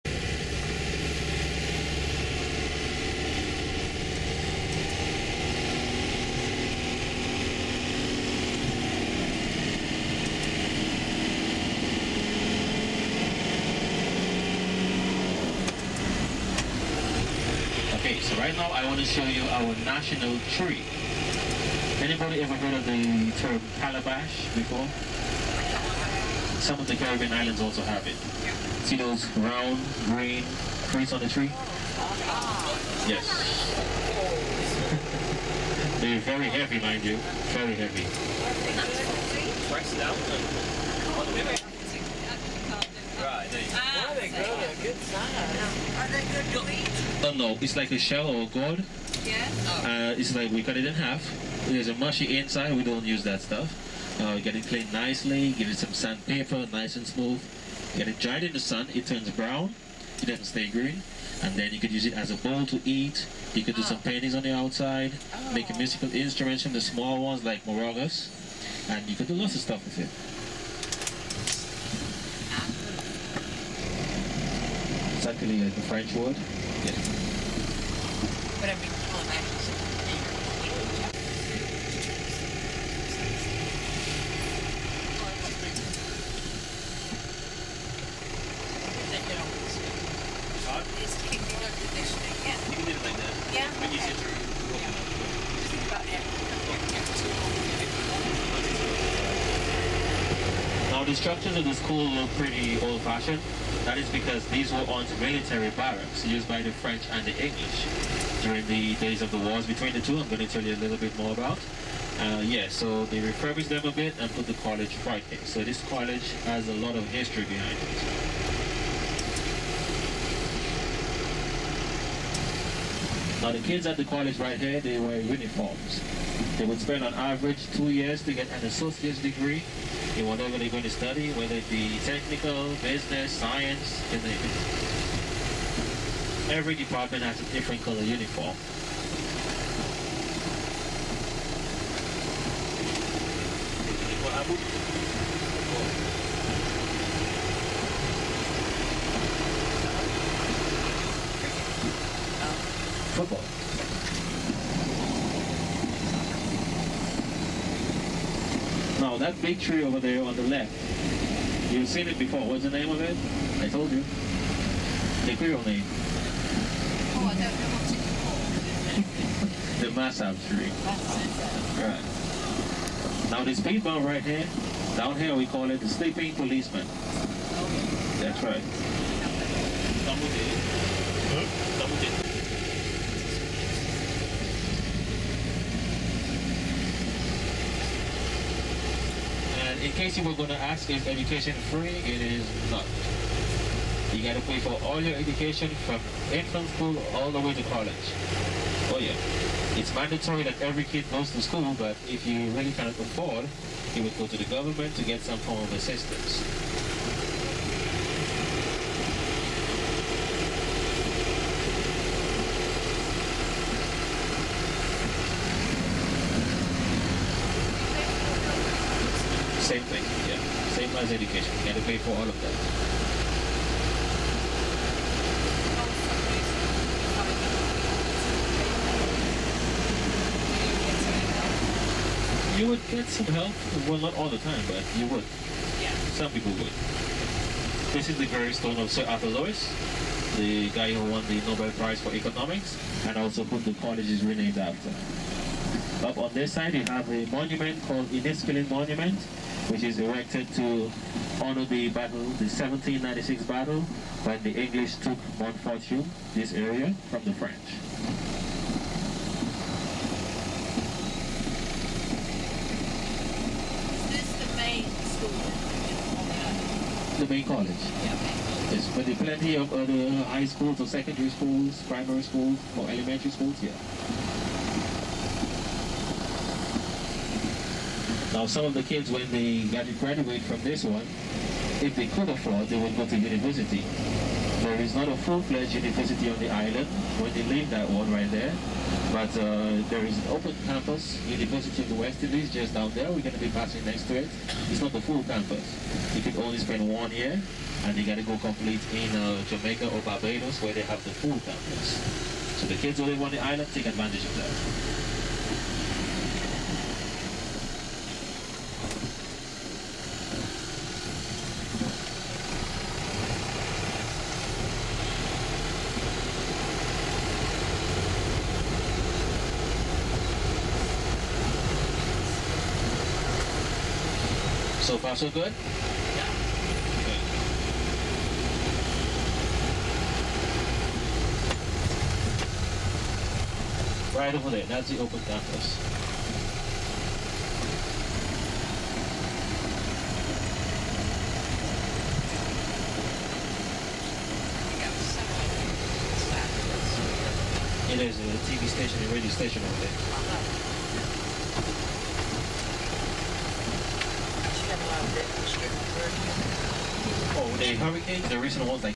driving through the campus of Sir Arthur Lewis Community College in Castries, St. Lucia
Sir Arthur Lewis Community College is the only such college on the Caribbean island nation of St. Lucia and as such is huge. This video is a Viking cruises tour through the campus which you can see is huge. The school is named after a Nobel Prize laureate from St. Lucia, Sir Arthur Lewis and the college was founded in 1985 and offers degrees in many fields. Uniforms are required and different for every school so one can tell which school simply by what the person is wearing. If the buildings look a bit strange for a community college campus, it is because they have been repurposed from old English and French army barracks--in my view an infinitely better use of them for education as opposed to oppression and violence. Somewhat surprisingly, tuition is not free and state support is limited. The date of this tour was March 2, 2018 during a Viking West Indies Explorer Cruise.